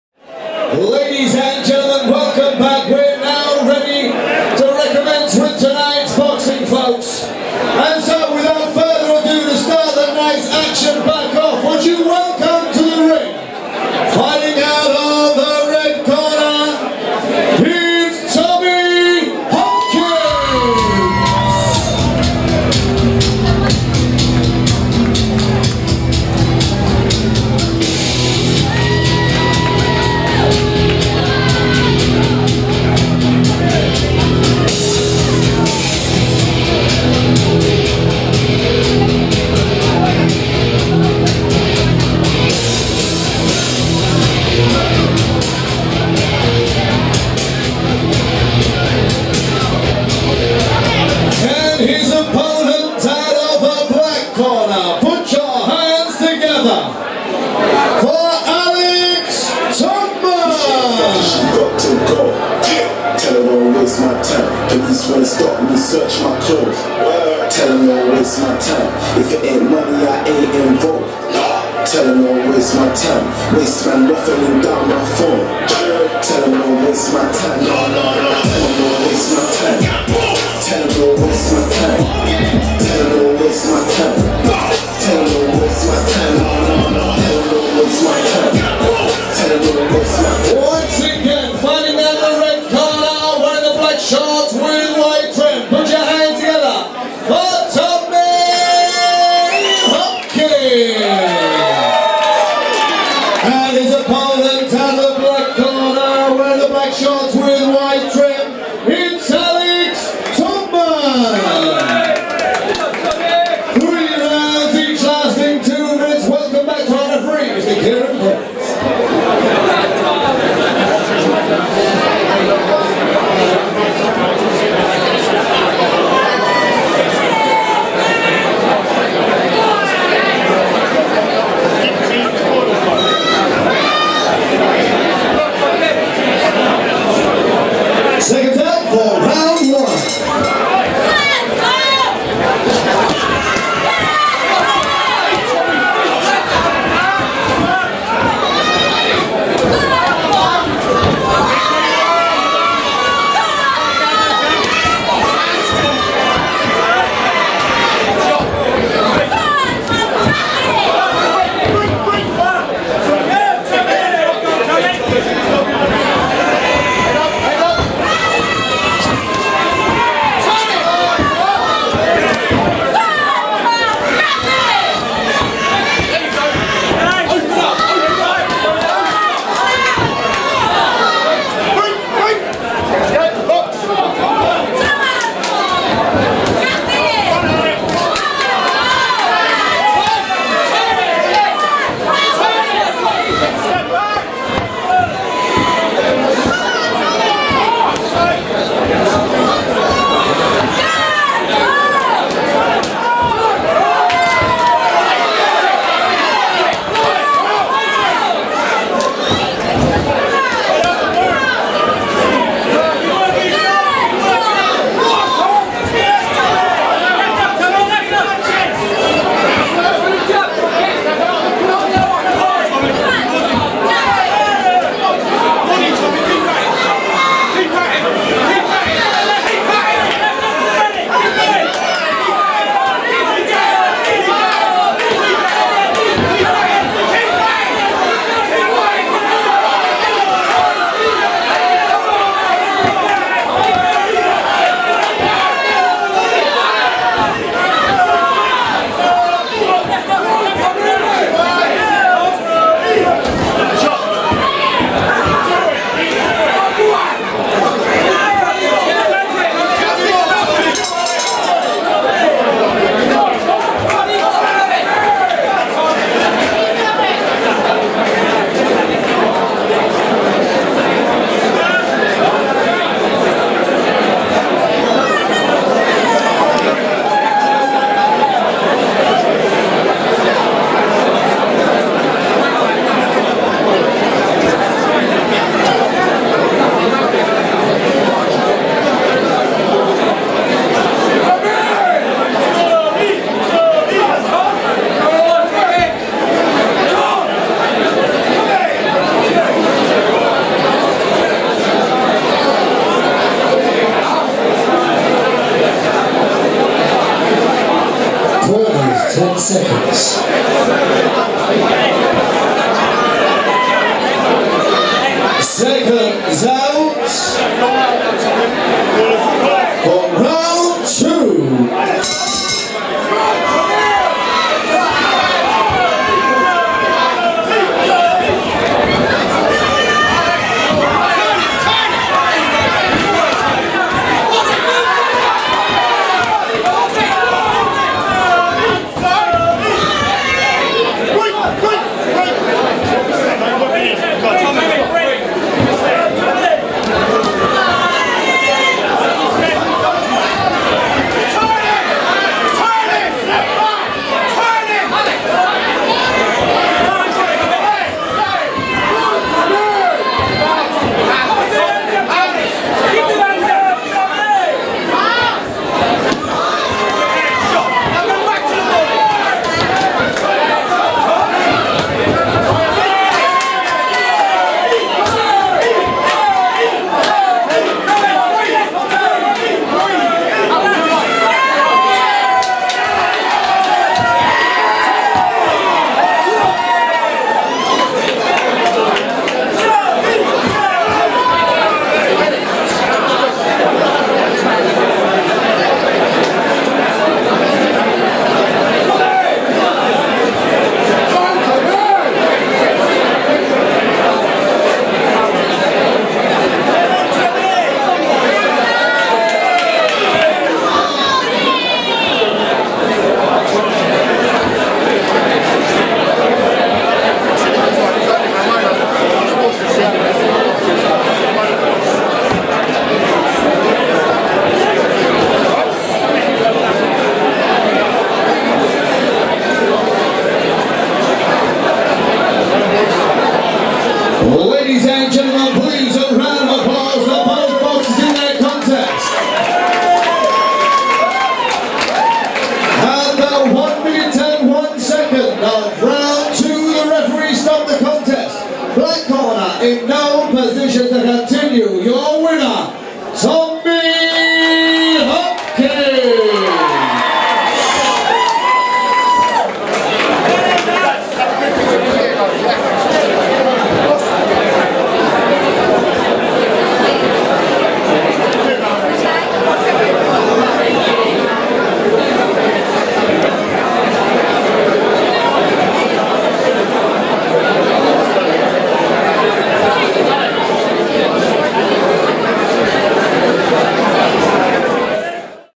Shush Promotions Tommy Hopkins Vs Alex Todman
Shush Promotions
Southpaw
Tommy Hopkins Vs Alex Todman
14th July 2017 at Shush Nightclub in Wantage